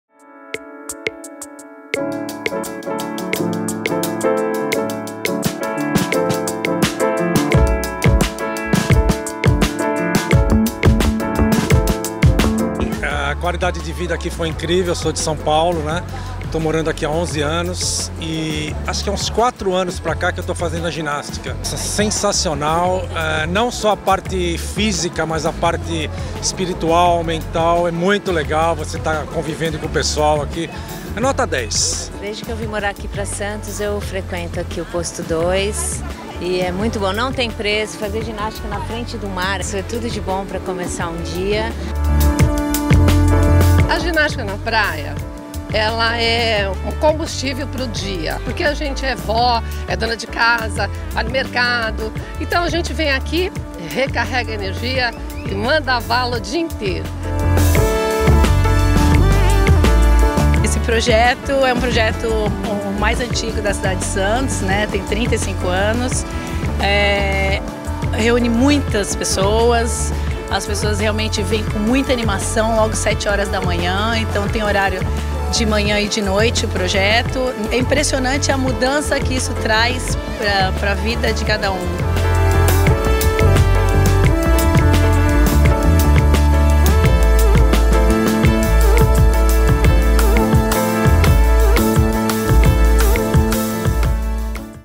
Ginástica na praia: há mais de 35 anos levando qualidade de vida aos santistas
Com mais de 35 anos, o  Programa Ginástica na Praia já atendeu milhares de santistas na orla, coma promoção de atividades físicas acompanhadas por professores. Se você se interessou e quer buscar mais qualidade de vida,  basta se inscrever no Posto 2, na praia da Pompeia, de segunda a sexta-feira, das 8h às 12h e das 14h às 18h. Mais informações pelo telefone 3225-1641.